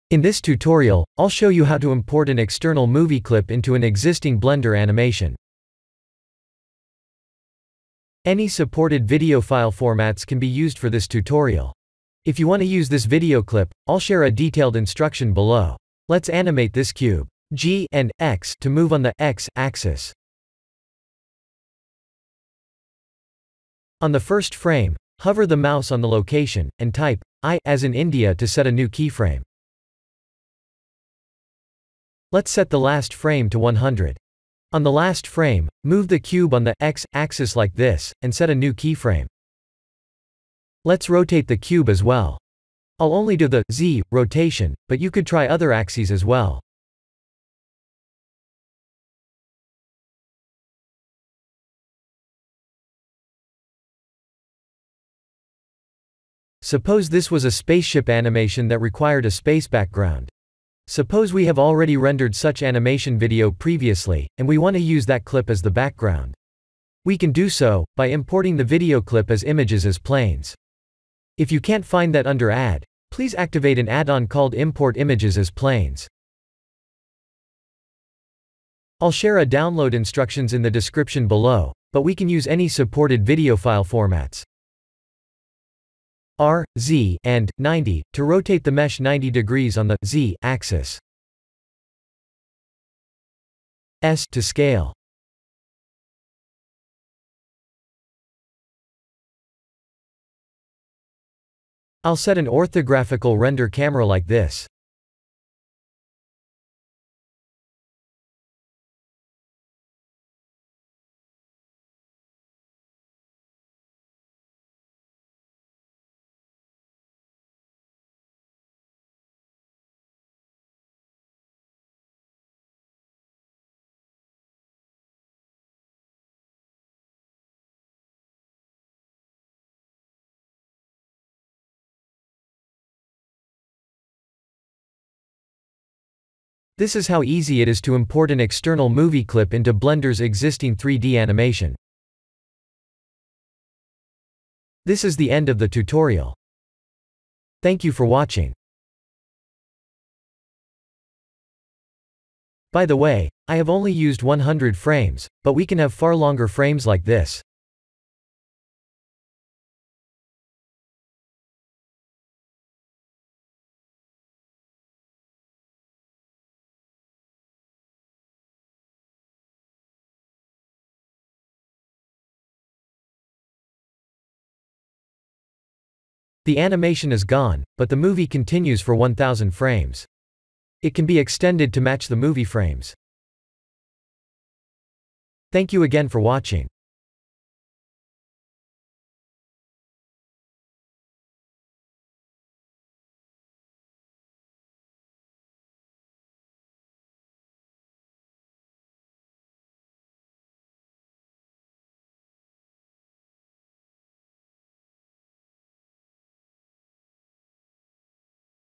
Blender Tutorial - Importing a movie clip
How to download and convert a webm video file

1. The star field animation can be downloaded on this
2. Click anywhere on the animation to change the direction, and click 'REC'.
3. After recording a clip, click 'REC' once again to finish.
4. Save the .webm video file.
5. , to conver the webm file to Mpeg.
6. Webm is not supported by Blender.  Although we can import '.webm' files as an image plane, but we'll only see a purple image when we play the animation.